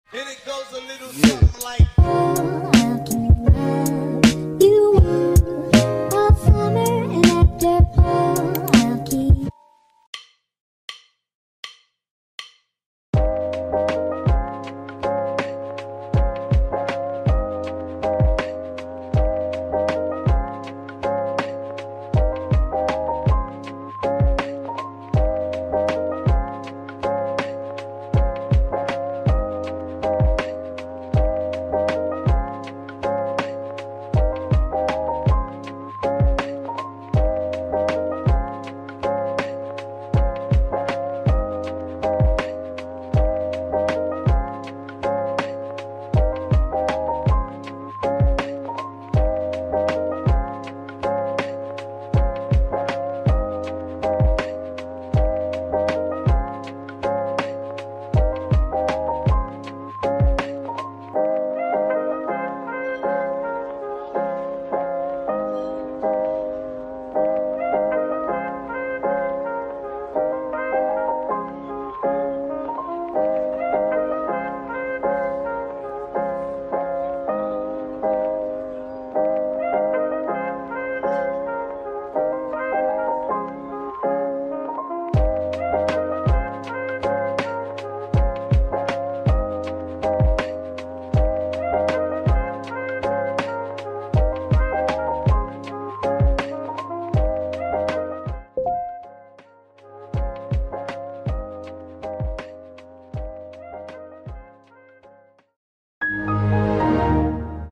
m o o d b o a r d c h a l l e n g e : p t 2
2nd Mood Board drawing! I've already finished a lot of the drawings but haven't edited the videos yet bcs I'm lazy, lol. Anyways, ty for watching!